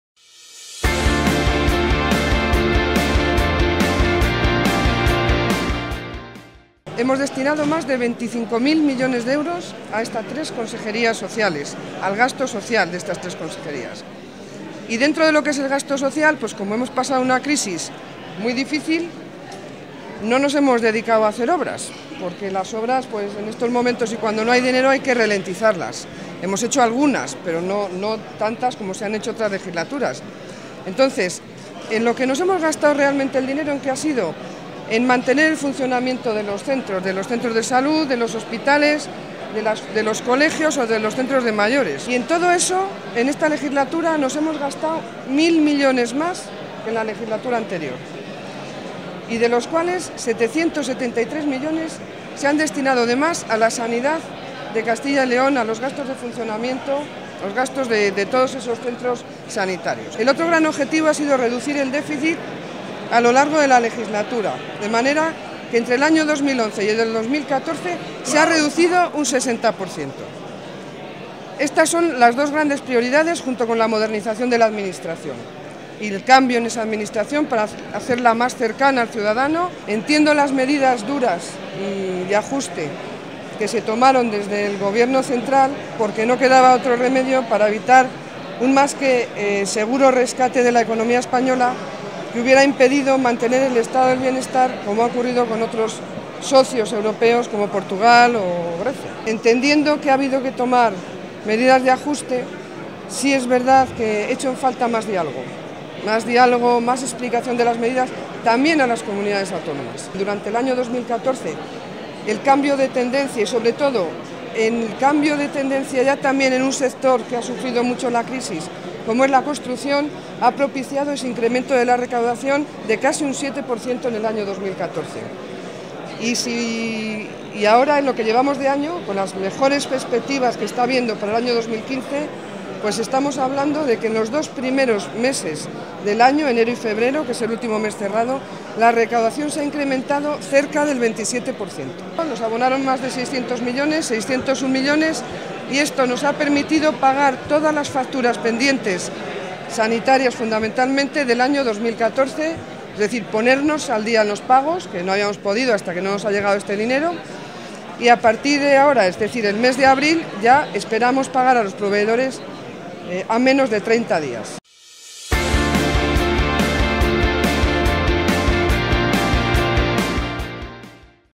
La consejera de Hacienda hace balance de legislatura en materia de Hacienda en Segovia
La consejera de Hacienda, Pilar del Olmo, ha hecho balance este viernes en Segovia de una legislatura “marcada por la dureza de la crisis económica, la más difícil que hemos vivido”, que el Gobierno del PP de la Junta de Castilla y León ha afrontado desde la convicción de que sólo con unas cuentas públicas claras, solventes y saneadas se podía mantener la calidad de los servicios esenciales. Este objetivo se ha conseguido, como acreditan los expertos, y alcanzarlo ha sido posible porque en los últimos cuatro años la Junta ha mantenido el gasto en las tres consejerías sociales.